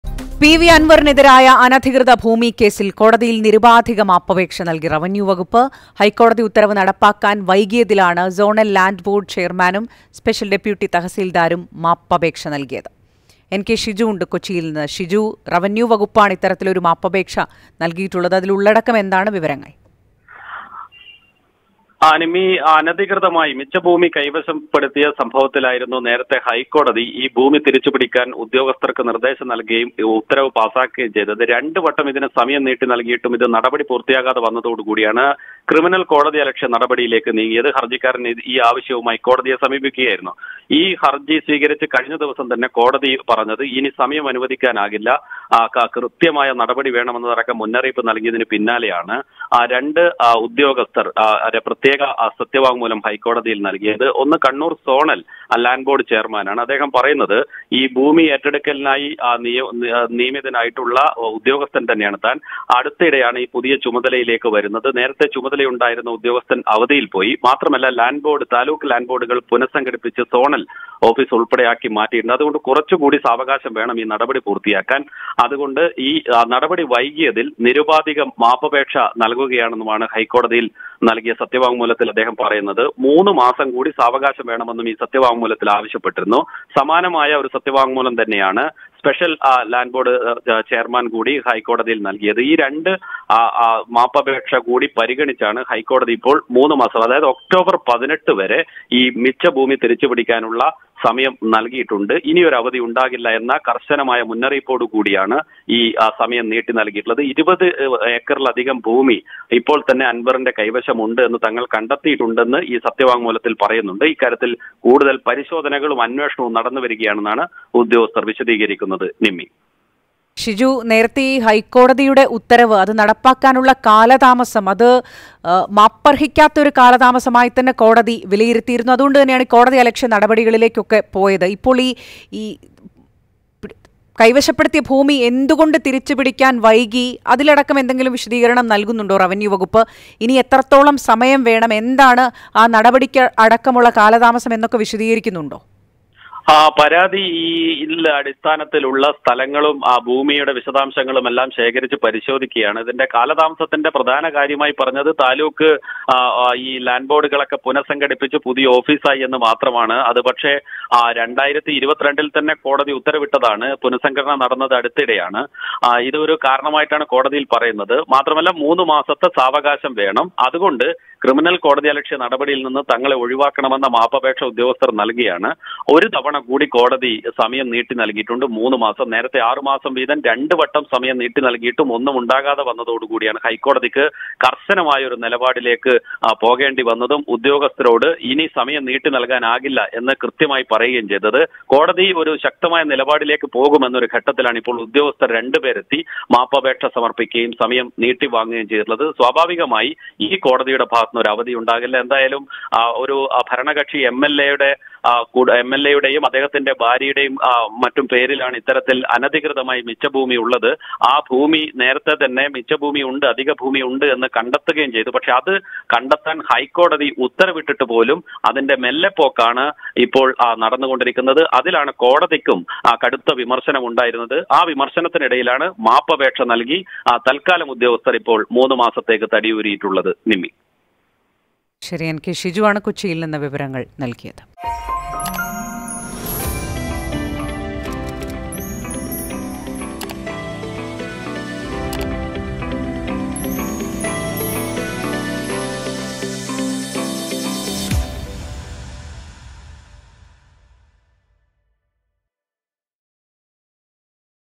പി.വി അൻവറിനെതിരായ അനധികൃത ഭൂമി ഇടപാട് കേസ്;കോടതിയിൽ മാപ്പപേക്ഷ നൽകി റവന്യു വകുപ്പ്|PV Anwar
പി.വി അൻവറിനെതിരായ അനധികൃത ഭൂമി ഇടപാട് കേസ്; ഹൈക്കോടതിയിൽ നിരുപാധിക മാപ്പപേക്ഷ നൽകി റവന്യു വകുപ്പ്
PV Anwar | Revenue Department